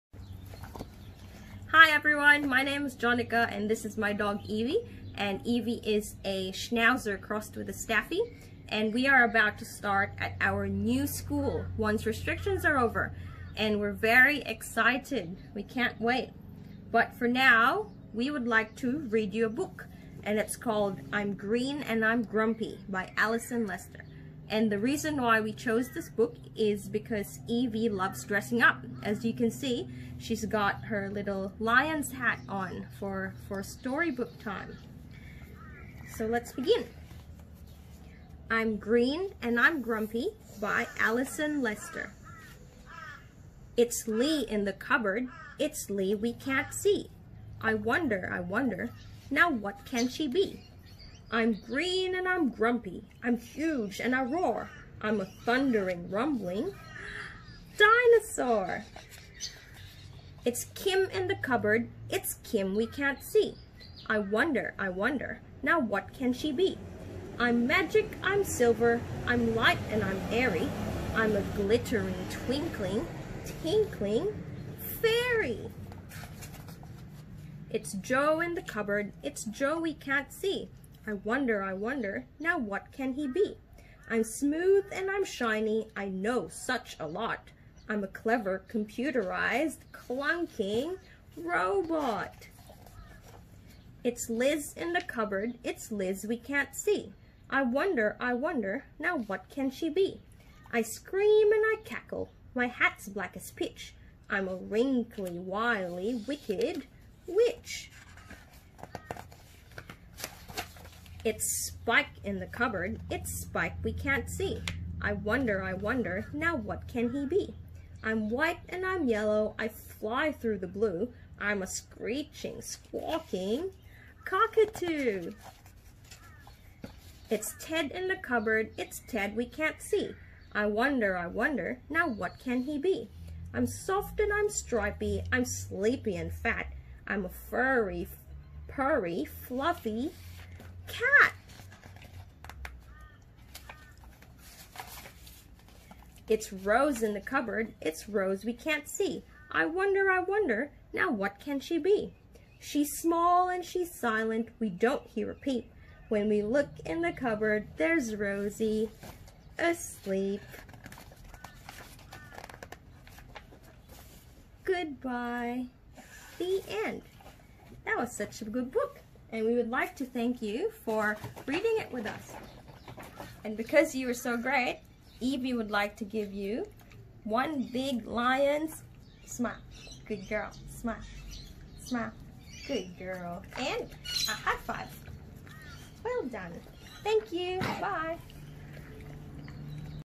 Read 'I'm Green and I'm Grumpy' with Eevee the Story Dog
Jonika and Eevee the Story Dog read 'I'm Green and I'm Grumpy'